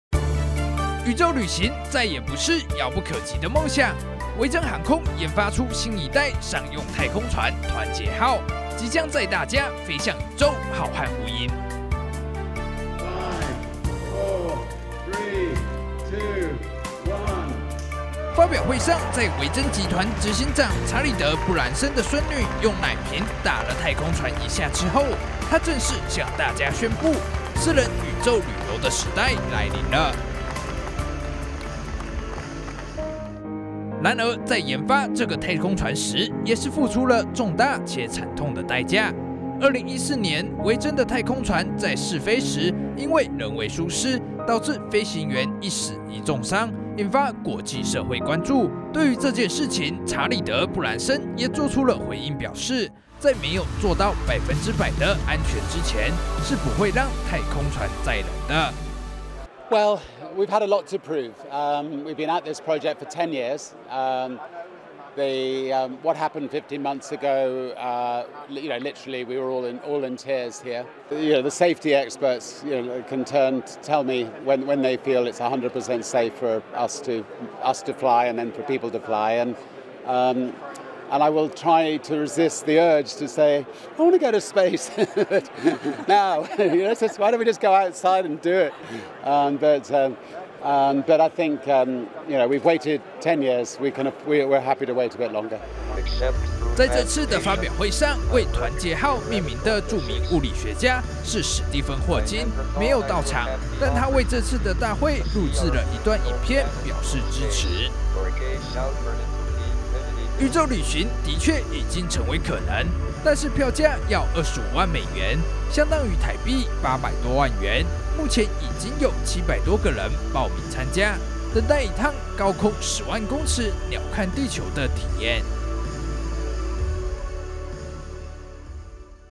新維珍太空船亮相 已700人預約遊太空【大千世界】宇宙旅行｜維京銀河｜史蒂芬霍金｜太空船｜太空科技
【新唐人訊】玩具總動員裡面的巴斯光年，曾經說過，「飛向宇宙，浩瀚無垠！」這句話可能不只是一句口號而已，現在要離開地球表面已經不再是夢想。維珍銀河新推出的「太空船二號」即將進入試飛階段！